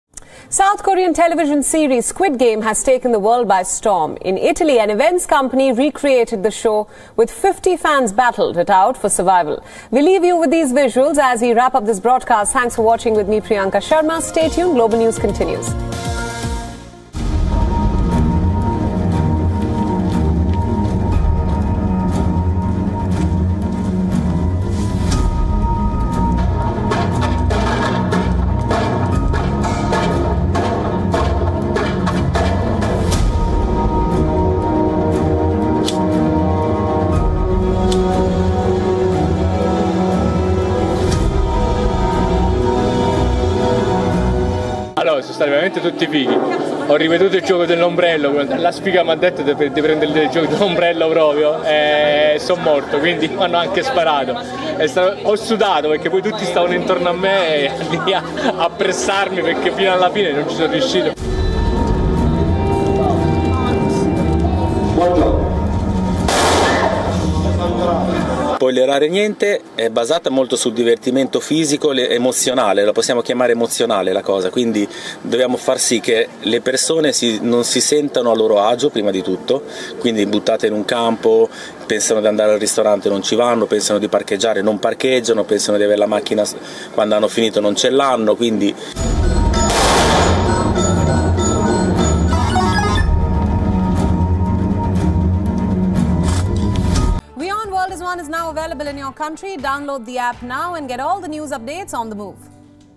Watch: Italy recreates real-life Squid games | South Korean Movies | Entertainment | News
South Korean television series Squid Game has taken  the world by storm. In Italy, an events company recreated the show, here are the visuals.